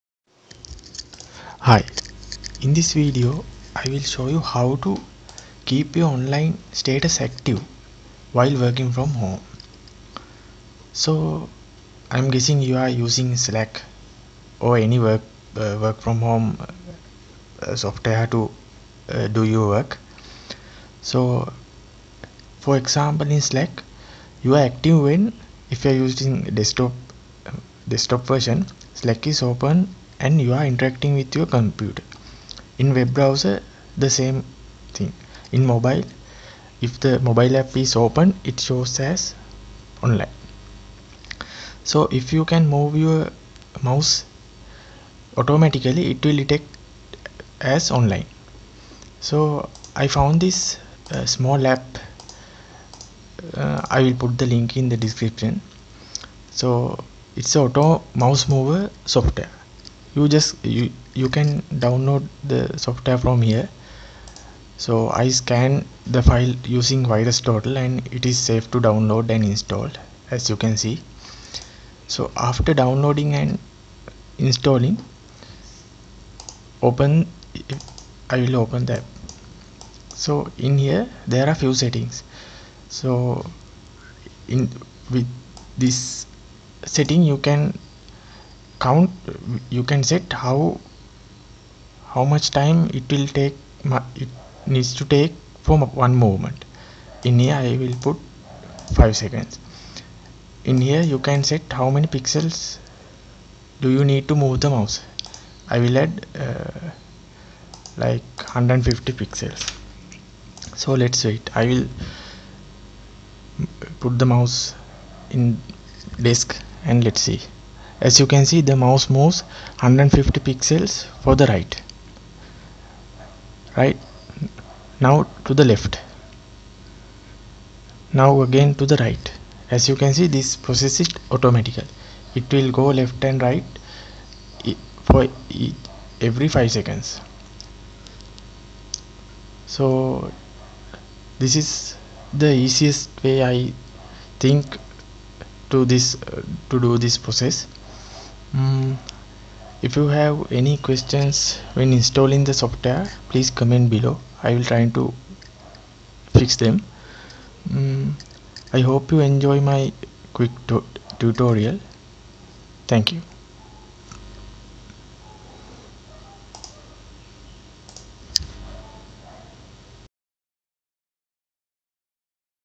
Make Slack Online Status Always Active | Working From Home Presence
In this video I will show you how to keep active in slack if you're working from home. This app can use in windows machines and can use with any app to stay active .Bitrix24 ,Hubstaff, Chanty or any app, this will work..